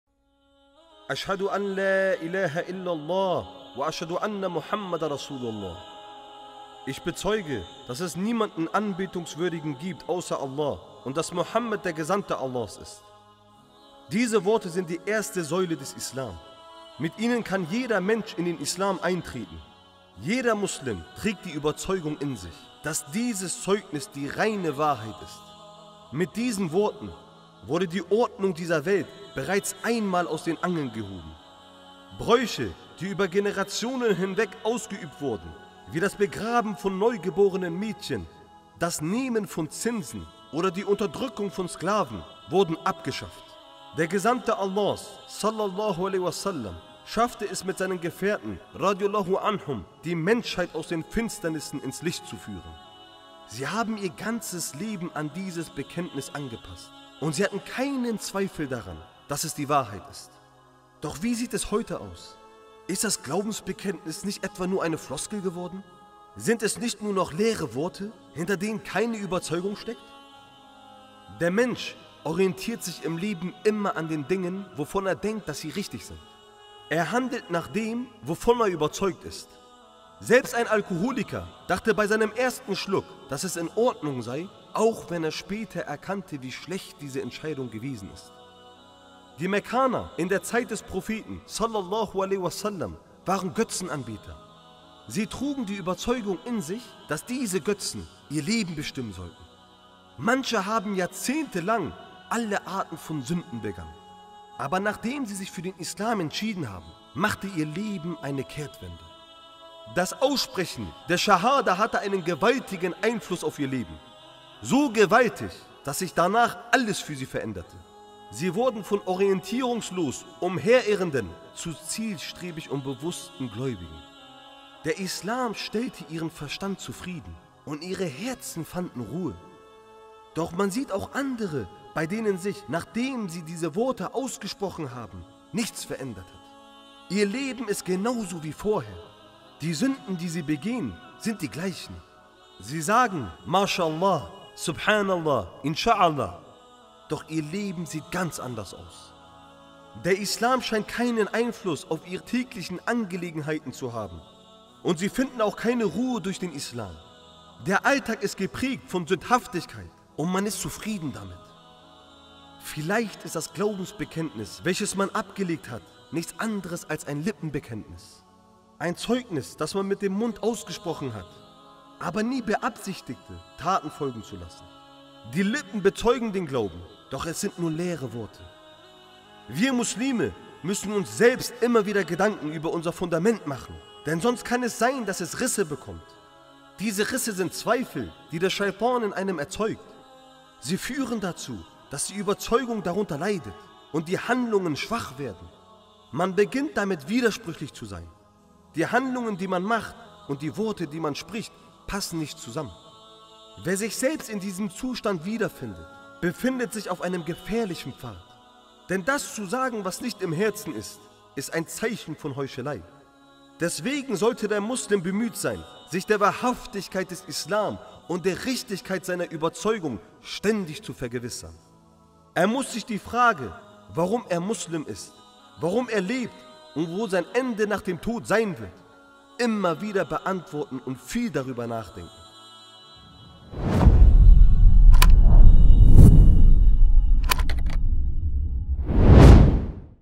Folgen Taten auf meine Worte? ᴴᴰ ┇ Worte zum Nachdenken┇ BDI
Vielleicht ist das Glaubensbekenntnis, welches man abgelegt hat, nichts anderes als ein Lippenbekenntnis. Ein Zeugnis, dass man mit dem Mund ausgesprochen hat, aber nie beabsichtigte, Taten folgen zu lassen. Die Lippen bezeugen den Glauben, doch es sind leere Worte.